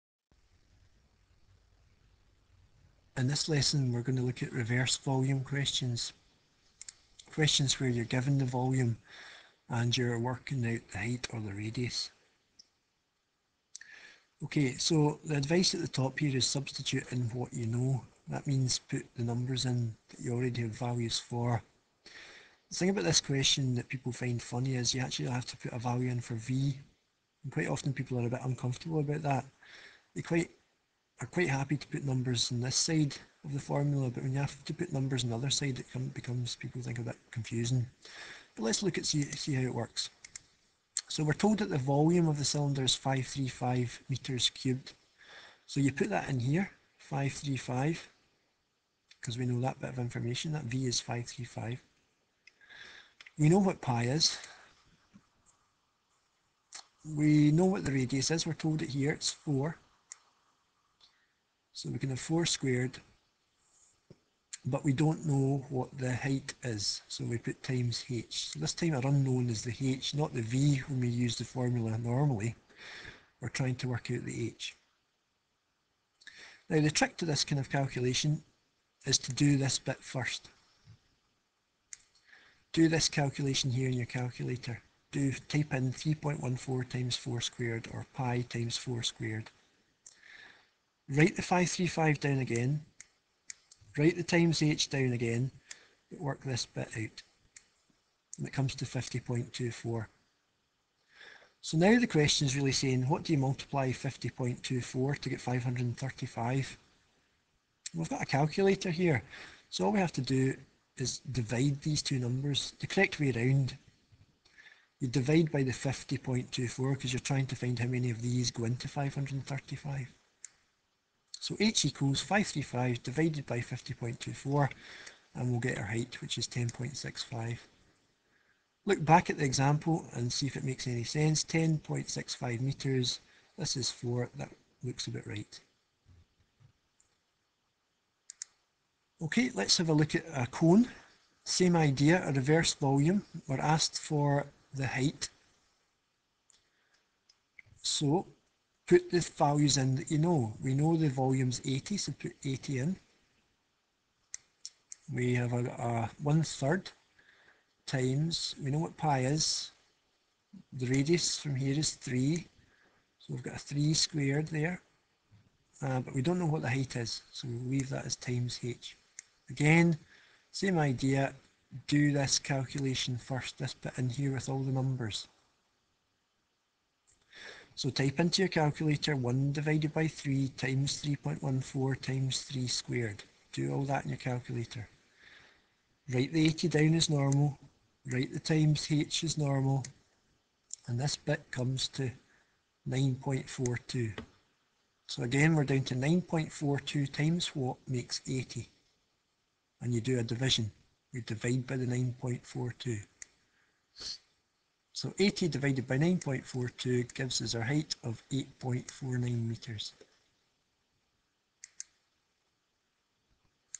How to deal with Reverse Volume questions
A Maths lesson that helps to find the value of the radius or height in volume questions.  I.e to reverse the calculation